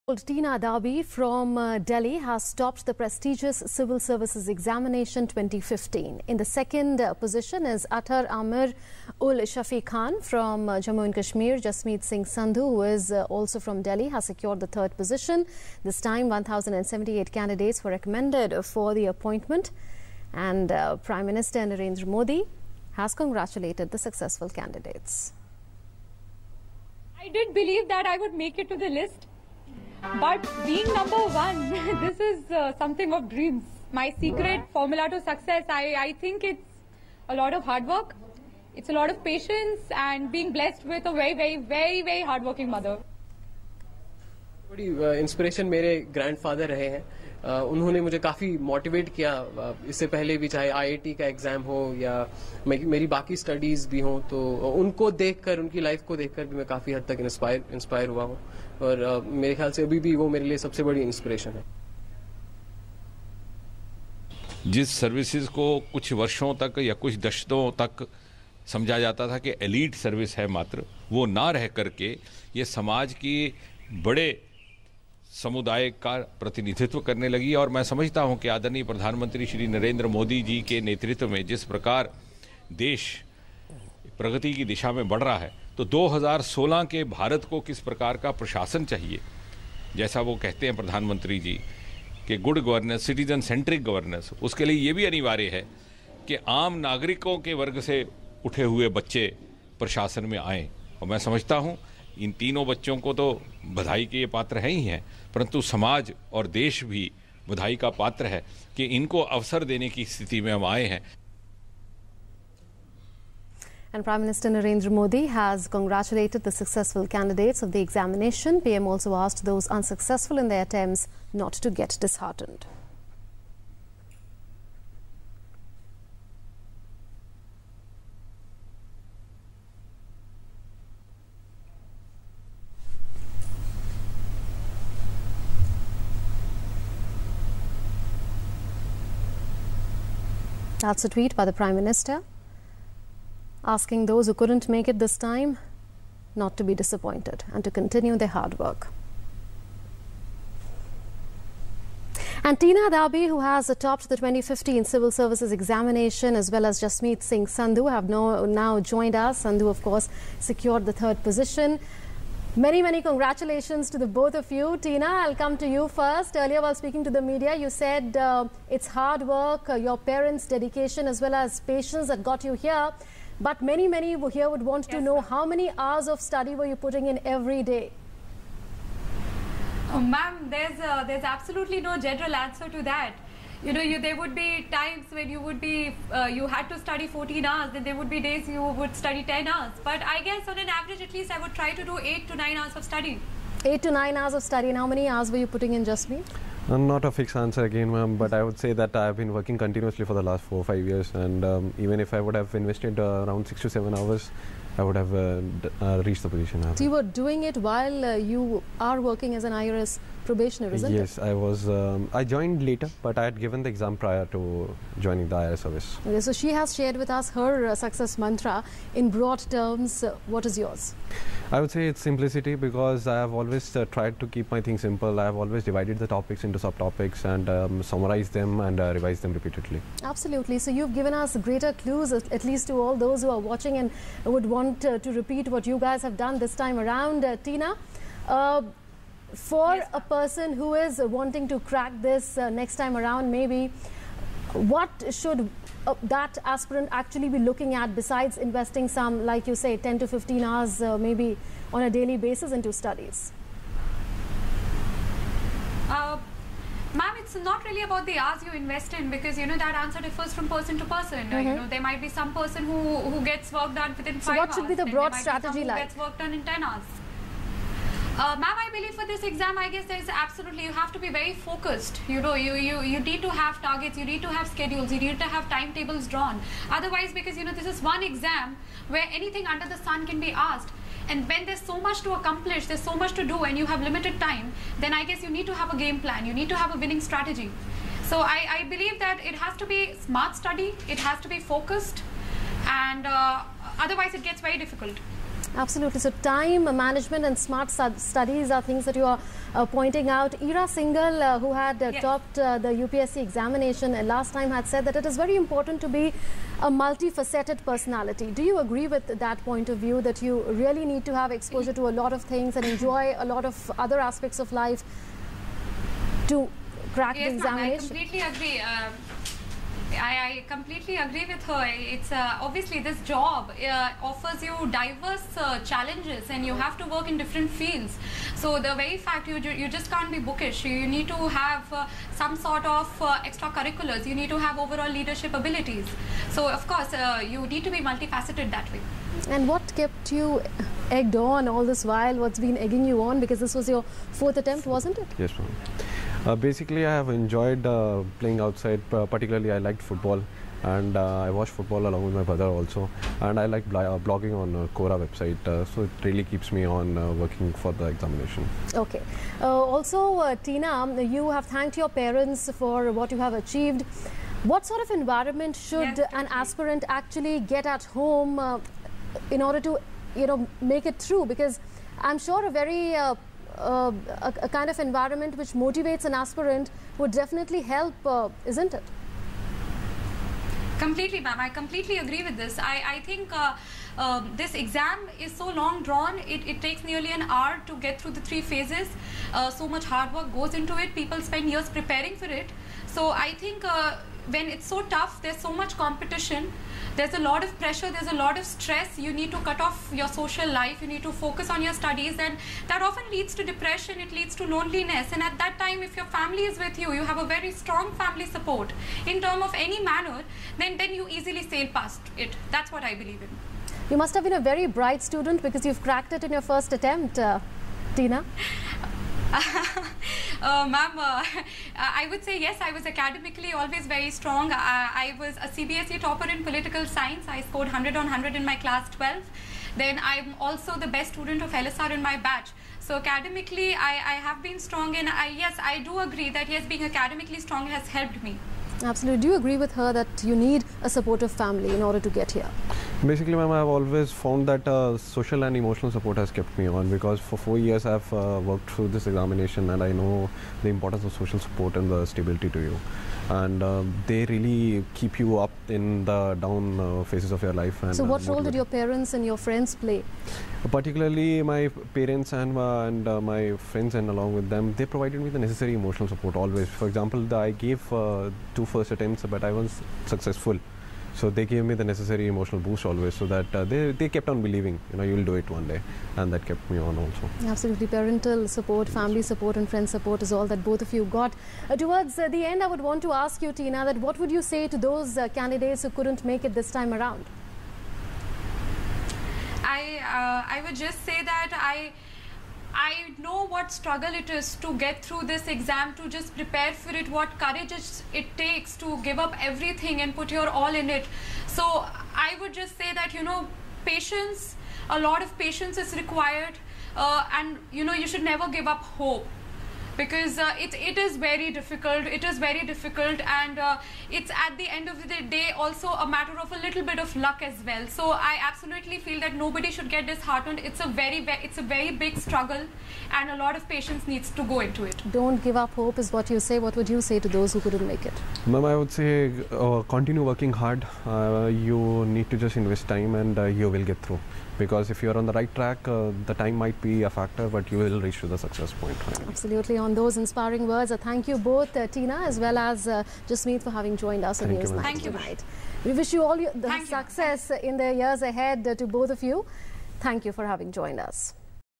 2015 Civil Service Exam result declared : Exclusive interview with Toppers
Twenty-two-year-old Tina Dabi from Delhi has topped the prestigious civil services exams 2015. In the second position is Athar Aamir Ul Shafi Khan from Jammu and Kashmir. Jasmeet Singh Sandhu, who is also from Delhi, has secured the third position. This time, 1,078 candidates were recommended for the appointment. Prime Minister Narendra Modi has congratulated the successful candidates of the exam.